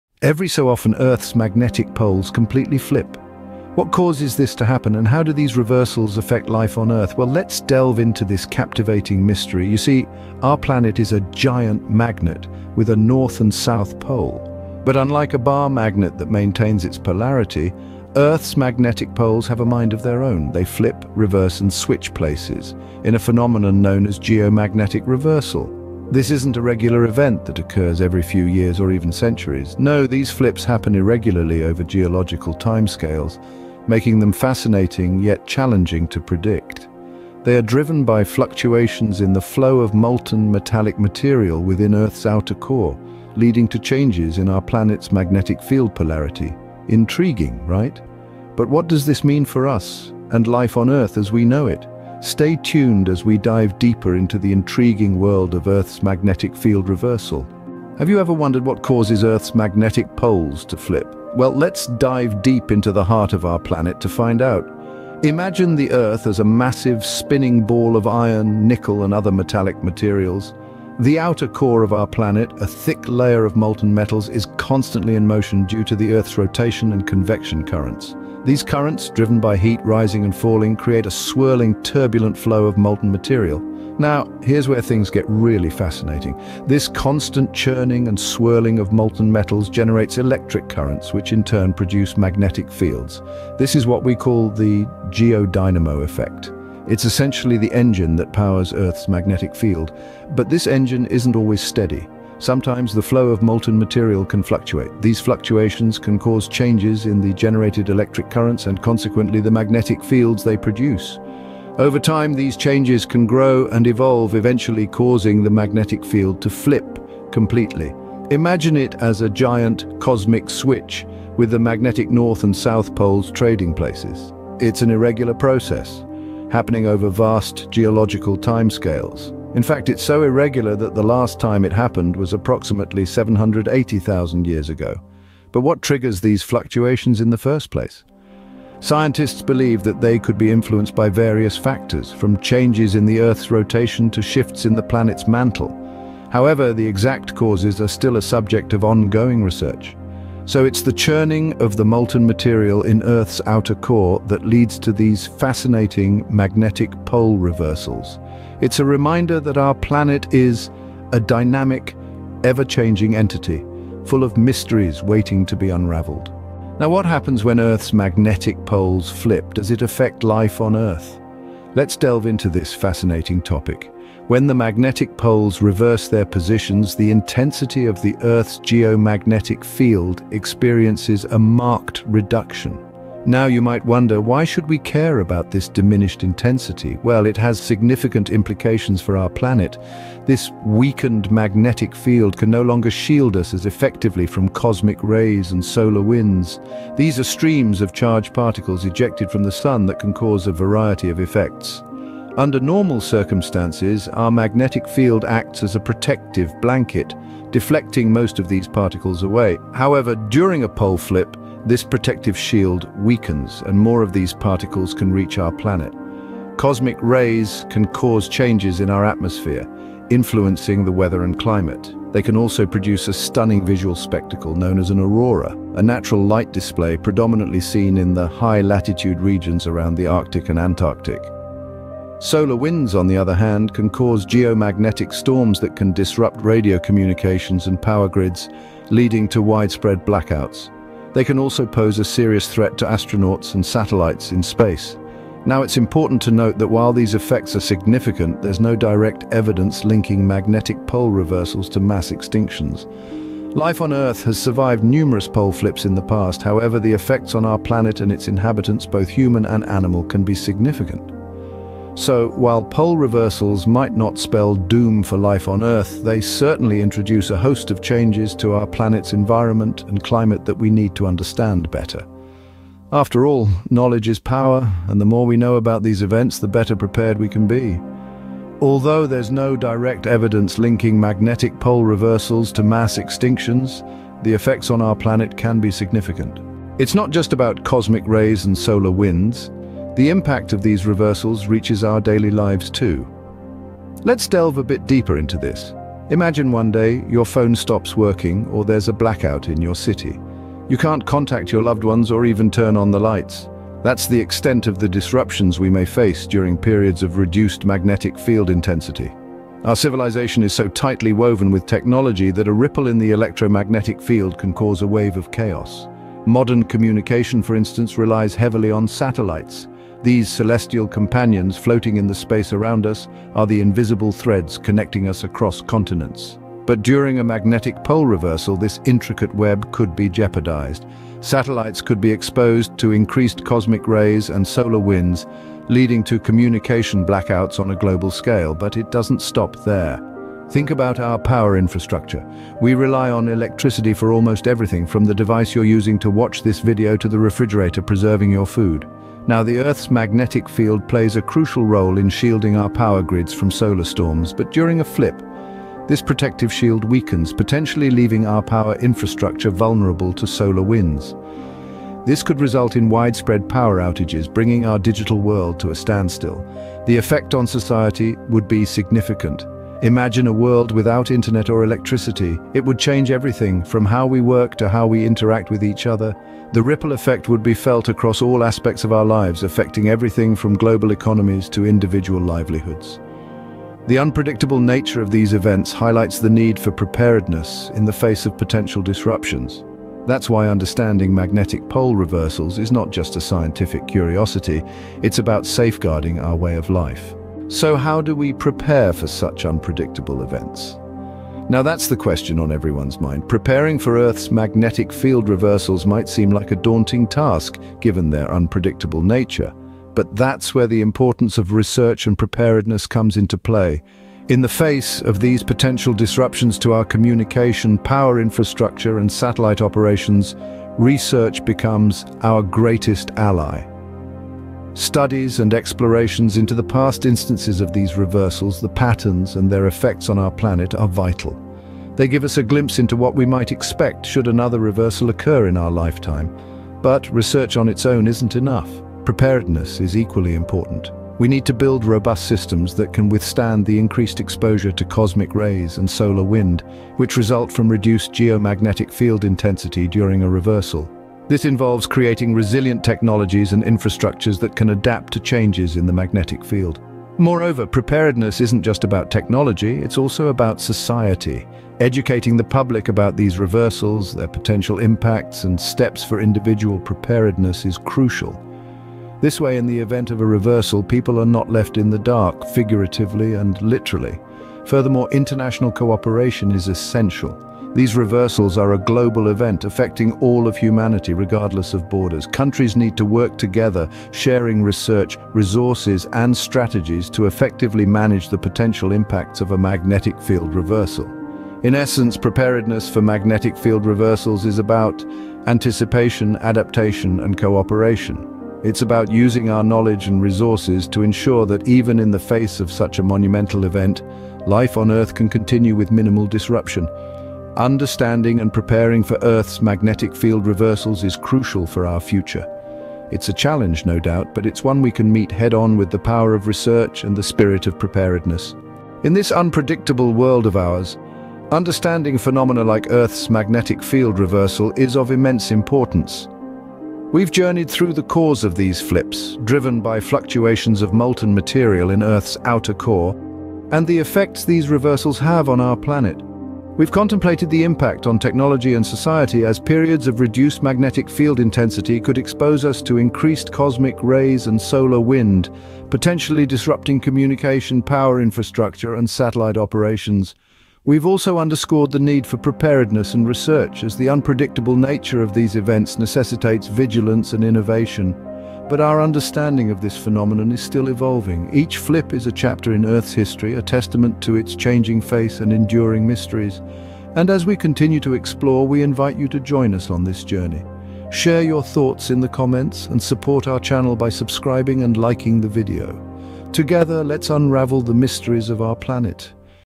The Flip Side: Earth's Magnetic Poles Reversal
Embark on a captivating journey as we explore the intriguing phenomenon of Earth's magnetic poles flipping. Delve into the underlying causes behind this enigmatic event and discover the potential impacts it could have on our planet, from its effects on life to its consequences for technology.

In this video, we'll delve deep into the science behind Earth's magnetic field, investigating the complex processes that drive its periodic reversals. Learn about the latest research and theories that attempt to explain this fascinating geophysical occurrence.

Prepare to be fascinated as we unravel the implications of a magnetic pole flip, from its influence on animal navigation and communication to its influence on our technological infrastructure. Gain a deeper understanding of this enigmatic process and its significance for our planet's future.

Don't miss this opportunity to expand your knowledge about the remarkable workings of our Earth.

OUTLINE:

00:00:00 Introduction
00:00:57 The Cause of Magnetic Poles Flip
00:02:46 Effects on Earth during Magnetic Poles Flip
00:04:46 Impact on Technology and Society
00:06:37 The Need for Preparedness and Research
00:08:38 Conclusion